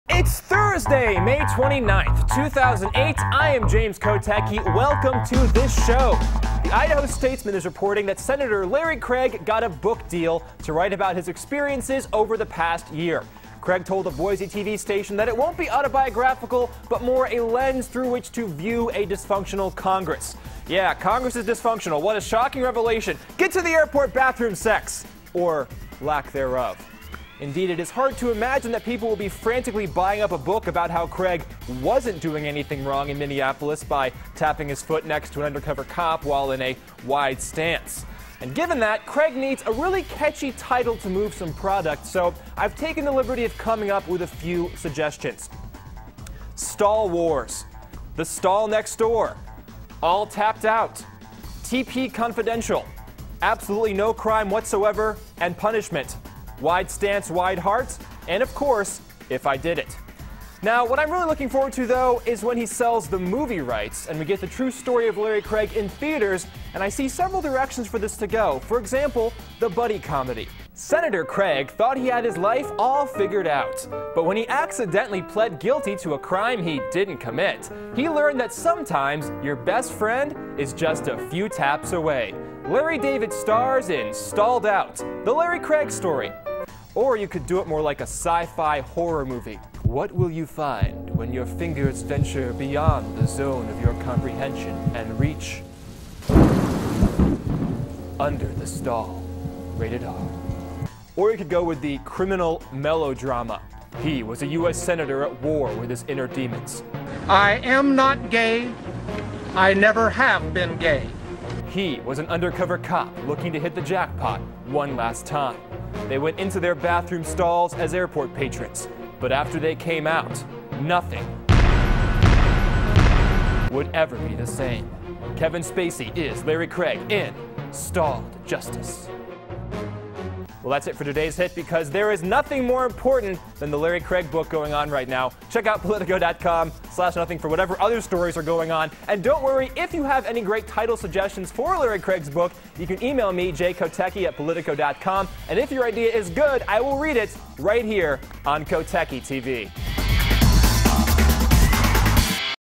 Larry Craig: The Movie!
Senator Larry Craig (R-Idaho) has a book deal to talk about his experiences in the past year.  Here are some title suggestions.

And what about the movie adaptation?